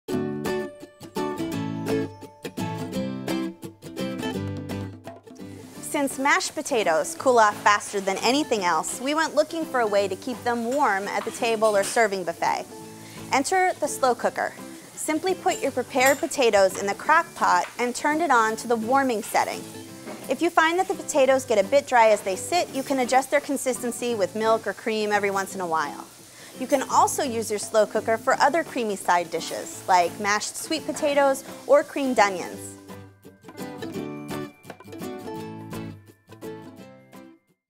How to Keep Mashed Potatoes Warm at the Table
You can trust the editors of Cook's Illustrated—experts of all things Thanksgiving—to deliver recipes that work.

Getting everything to the table on Thanksgiving can be challenging. What's even more challenging is getting everything to the table warm. Here's a tip for keeping your mashed potatoes warm while the rest of the meal cooks.

USE A SLOW COOKER AND SET IT TO LOW.

Creamier thick side dishes such as mashed potatoes or candied sweet potatoes—and even macaroni and cheese—will stay hot for an hour or more.